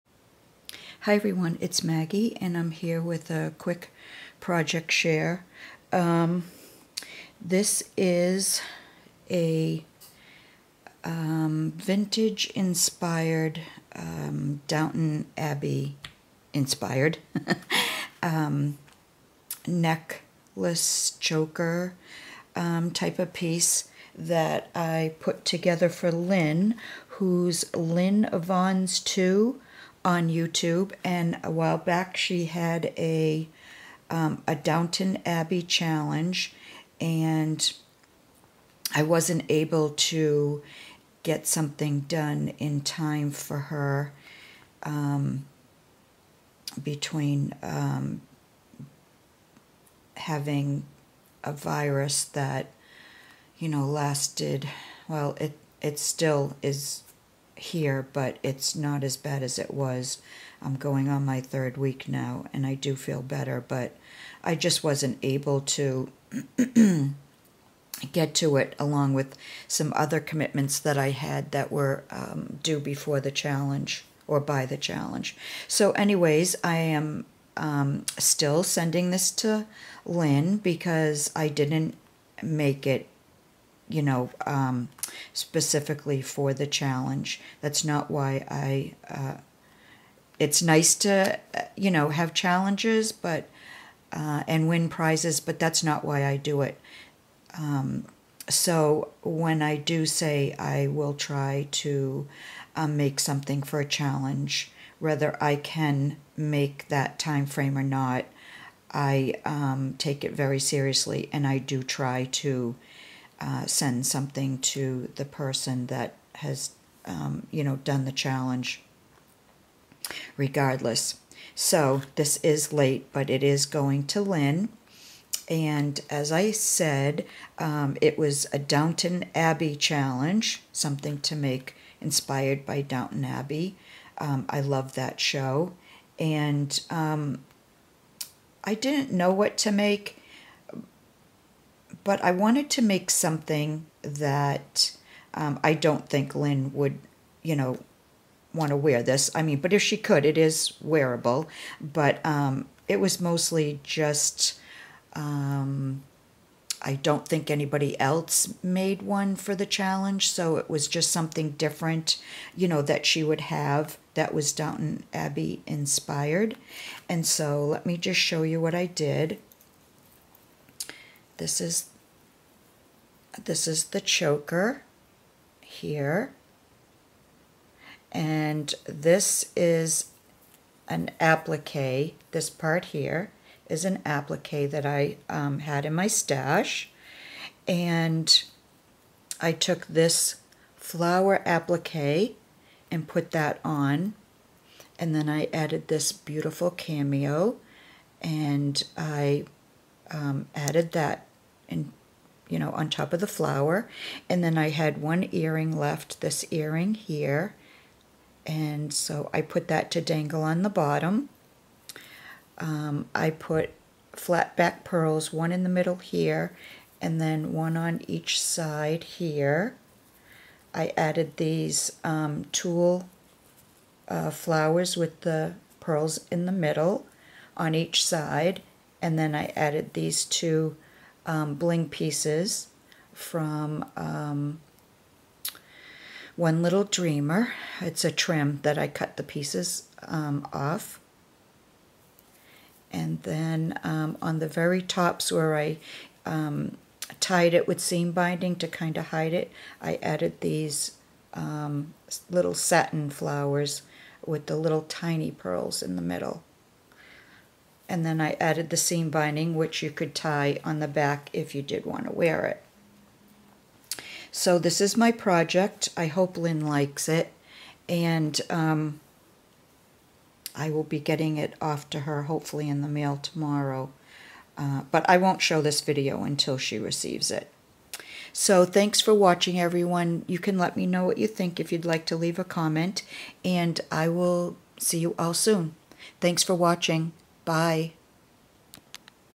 "Downton Abbey" inspired Necklace for Lynn - Project Share!
Hi everyone!  I wanted to share an applique necklace I made for Lynn who is lynnyvonnestwo here on utube.  I'll put the link below to her channel.